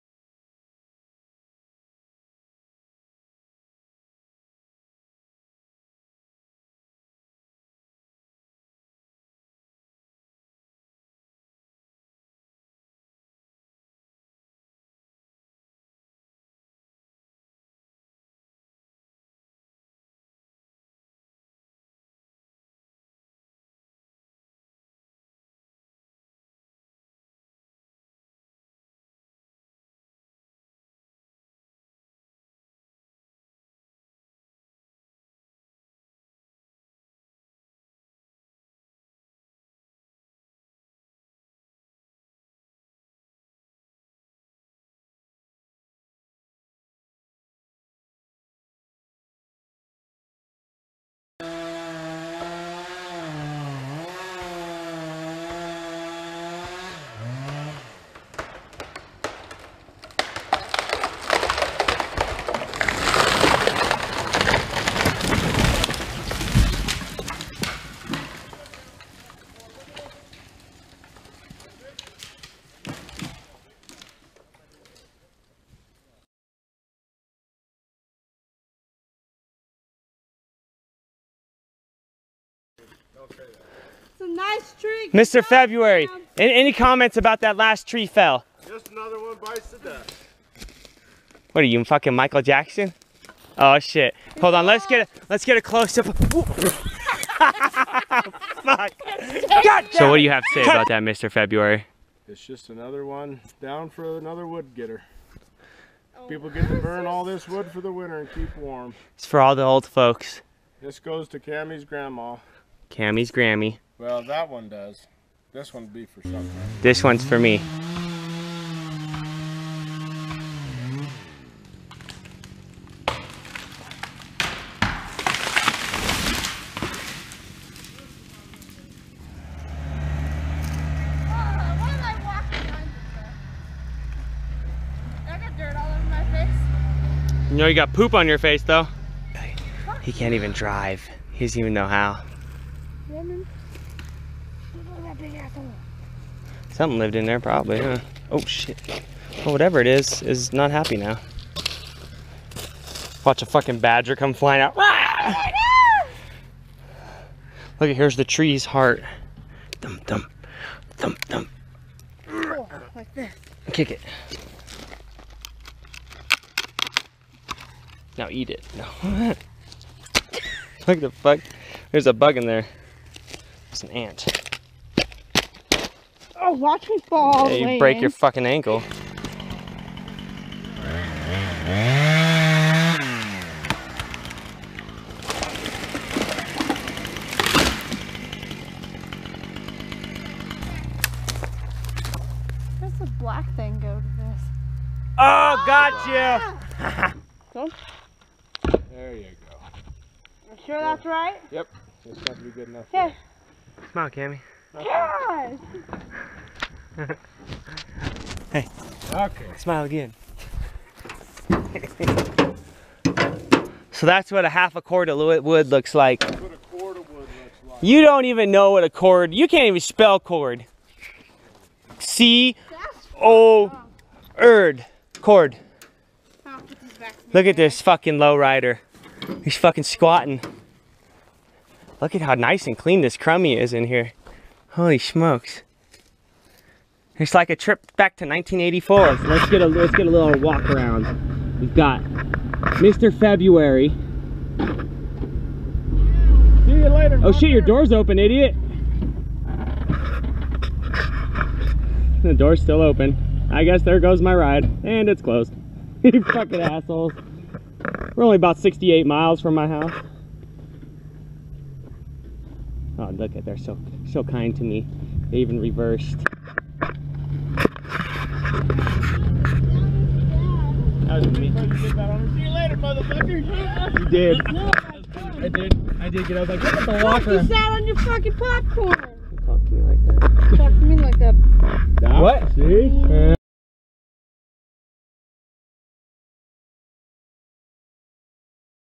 Cutting Wood with Mr.February Episode 2
On this episode we ended up bringing our good friend Patsy. We ended up dropping a 104 ft Lodgepole  Pine as well as a few others. I was running my Husqvarna 455 and Mr. February  brought out the trusty Stihl 044. Lots of fun as usual. More to come if you guys want them.